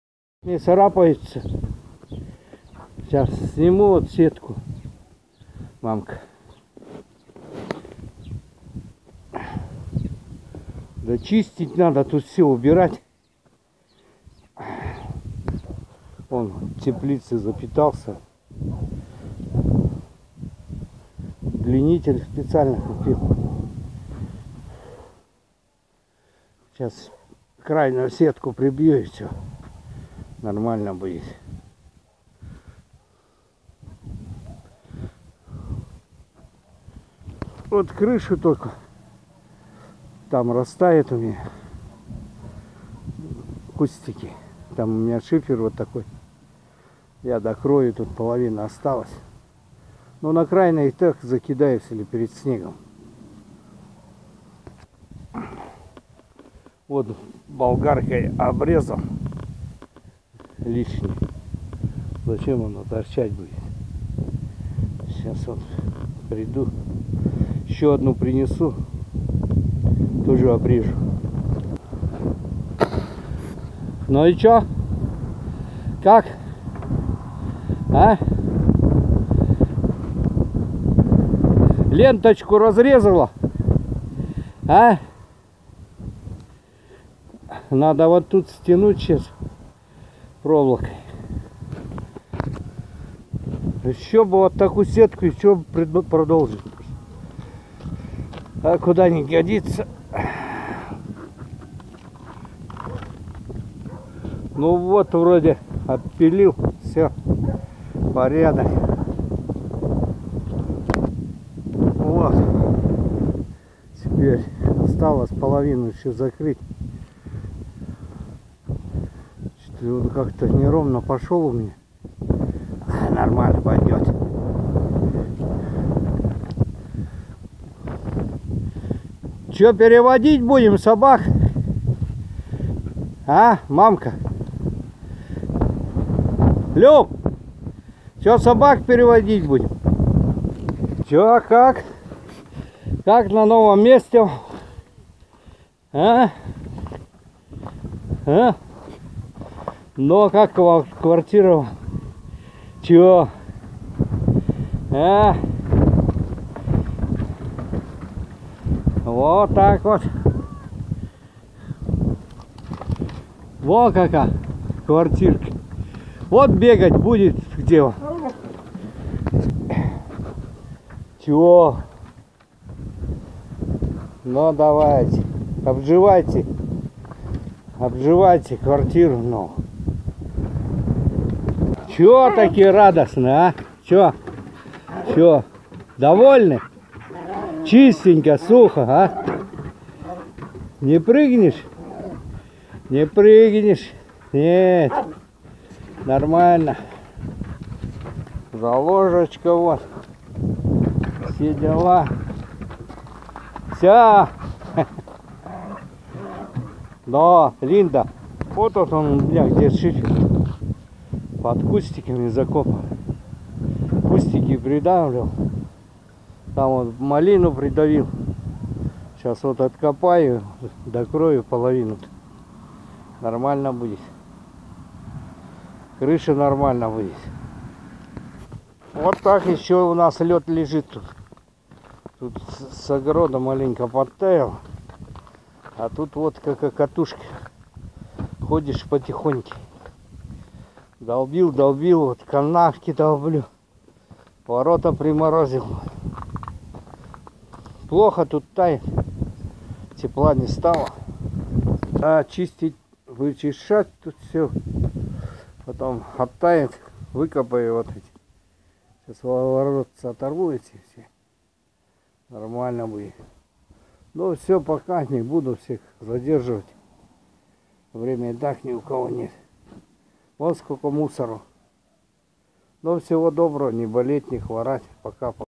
Все новая вольера ,готова .Правда с не доделками 😂
Насаждения  буду откапывать  ,тогда до делаю крышу а сейчас на время закрыл вторую половину крыши ,крупноволновым  шифером.На случай снега !!